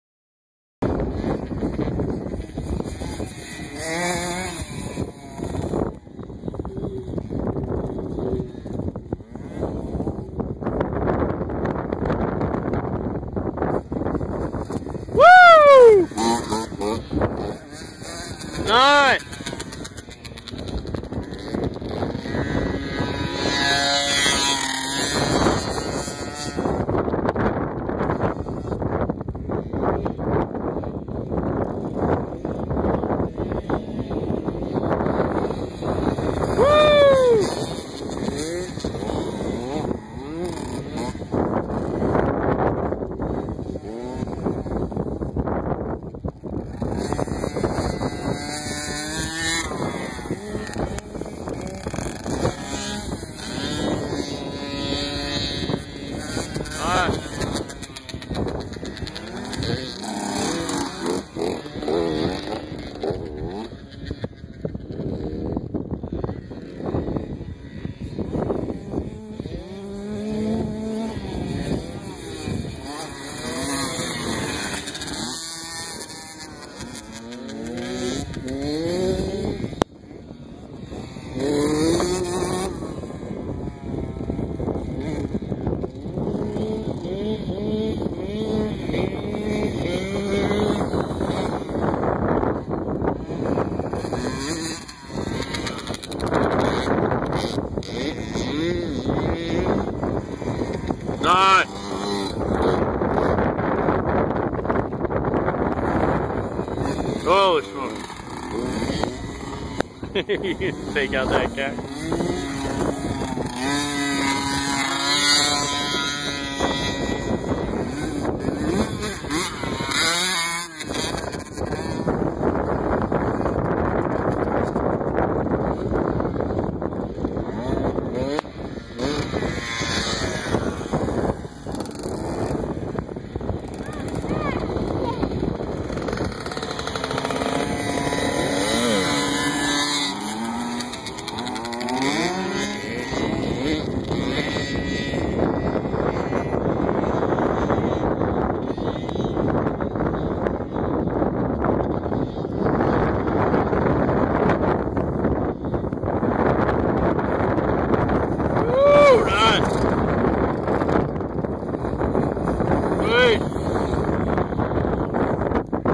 65's and 80 at Rainbow
The boys found a cool jump they couldn't stop hitting.